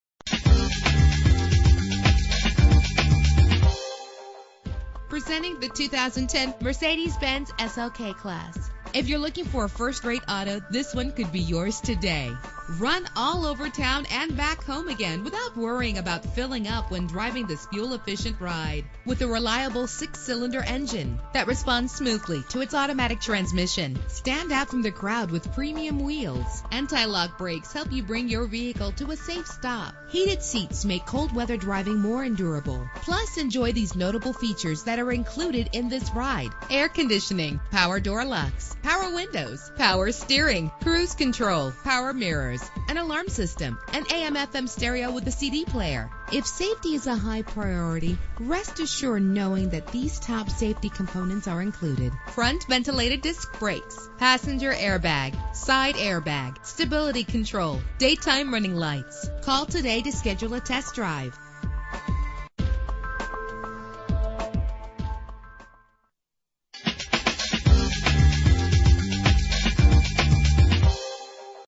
2010 Mercedes-Benz SLK-Class - San Jose CA
Year: 2010
Make: Mercedes-Benz
Model: SLK-Class
Trim: SLK300
Engine: 3.0 liter 6 cylinder 24 valve
Transmission: Automatic
Color: Mars Red
Mileage: 8
Address:
3000 E Capitol Expressway
San